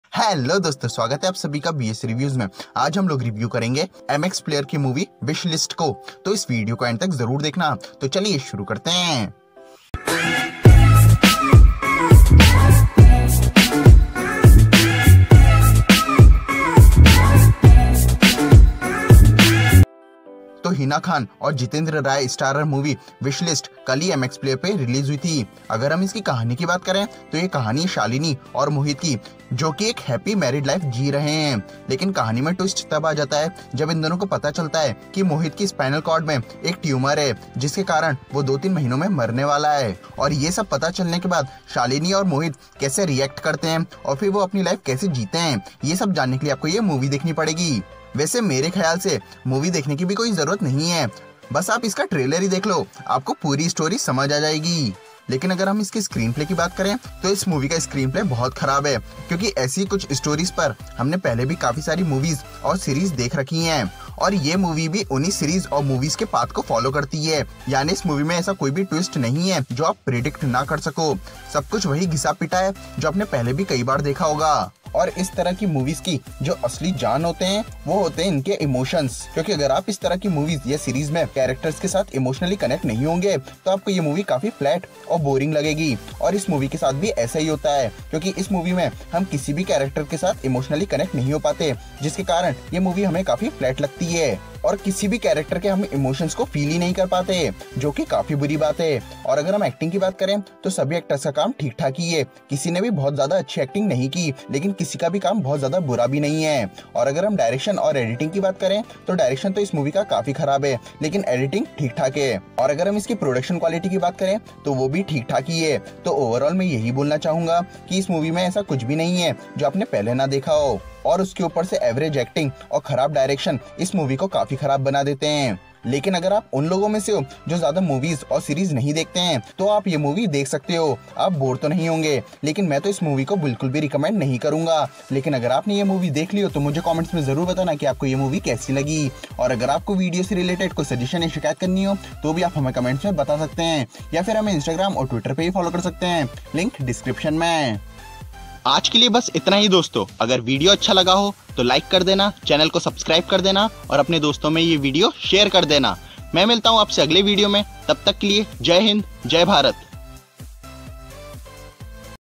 Wishlist Review! MX player Movie Wishlist Honest Review! Hina Khan! Jitnedra Rai !
MX player movie Wishlist Starring Hina Khan and Jitnedra Rai we are Reviewing today. In this video we talked about some good and bad points of the MX player movie Wishlist Starring Hina khan and Jitnedra Rai. So what are you waiting for go and watch the honest review of MX player movie wishlist and don't forgot to like, commment and subscibe our Channel 🙏🙏